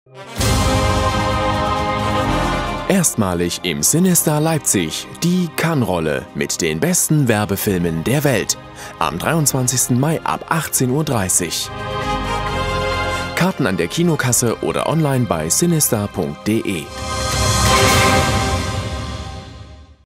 Cannes Rolle 2013 am 23.05.2014 in Leipzig
Werbung im Kino nervt? Das sehen wir anders. Lasst euch von den besten Werbespots aus dem Jahr 2013 überzeugen! Ob Bronze-, Silber- oder Gold-Löwengewinner -- MTP Leipzig zeigt exklusiv alle Highlights des internationalen Werbefestivals Cannes Lions erstmalig im Cinestar Leipzig. Angelehnt an die Oscars, das Pendant zur Prämierung in Cannes nur für die Filmbranche, lautet das diesjährige Motto „The Lions", welches einen glamourösen Abend verspricht.

Sichert euch einen der 360 Kinoplätze im Vorverkauf und erlebt einen einzigartigen Kinoabend mit Überraschungseffekt!

Wir freuen uns auf euch und auf den Moment, wenn es heißt: „And the Lion goes to..."!

Alle Daten auf einen Blick:

VVK-Stelle: CineStar Leipzig, auch online erhältlich
Ticketpreis: 8 Euro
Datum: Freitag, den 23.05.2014
Veranstaltungsbeginn: 18:30 Uhr
Location: CineStar Leipzig